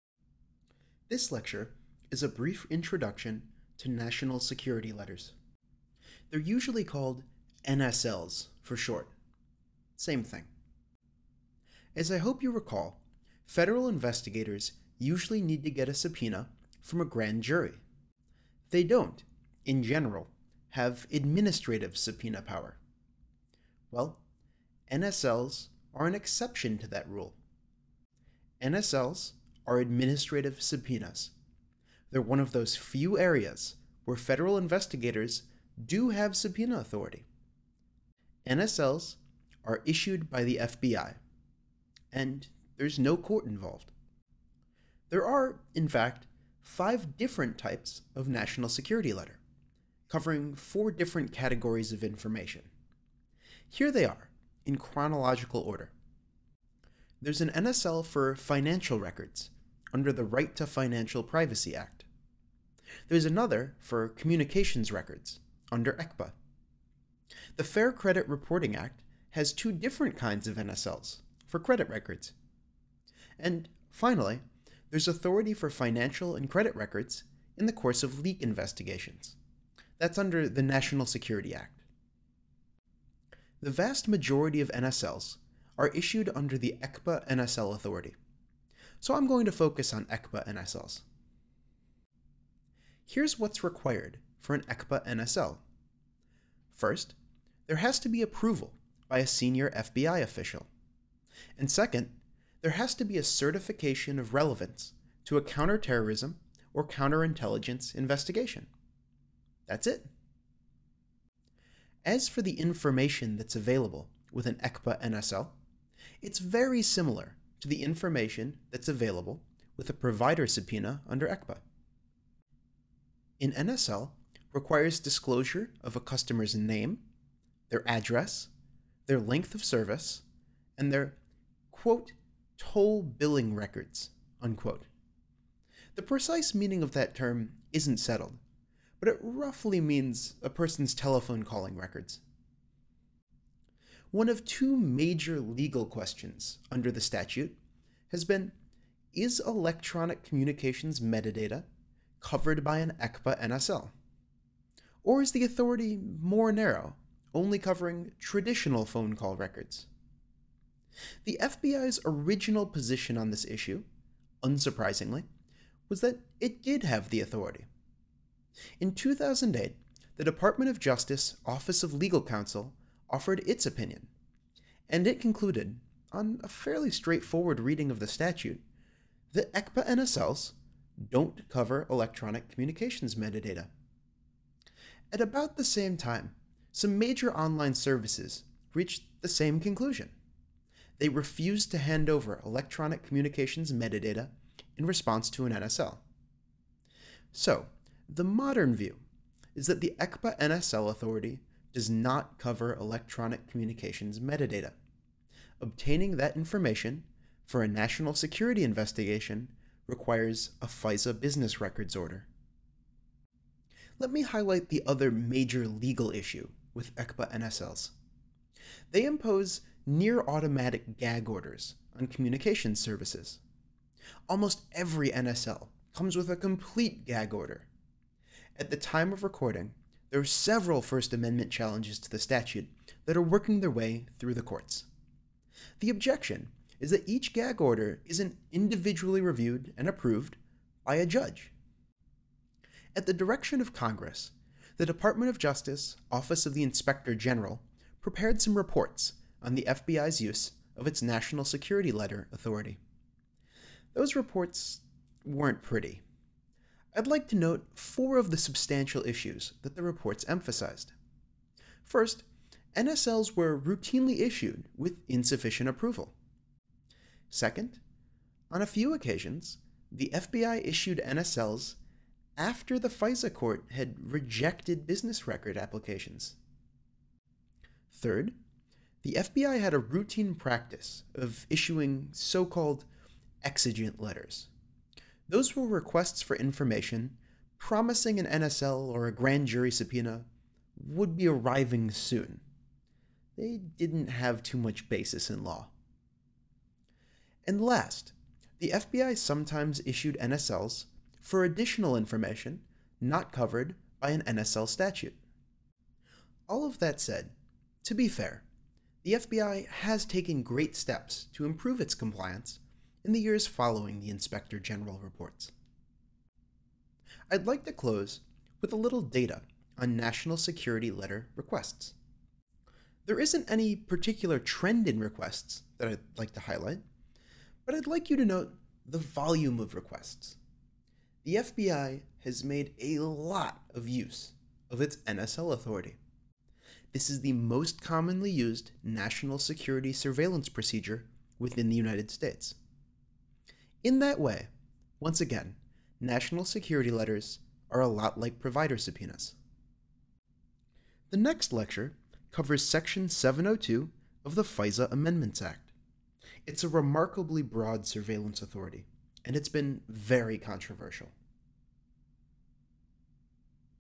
National Security Letters
Topics: National Security Letters, the Electronic Communications Privacy Act, 18 U.S.C. § 2709

Source: This video is archived from Surveillance Law, first offered by Stanford Law School on Coursera in Fall 2014.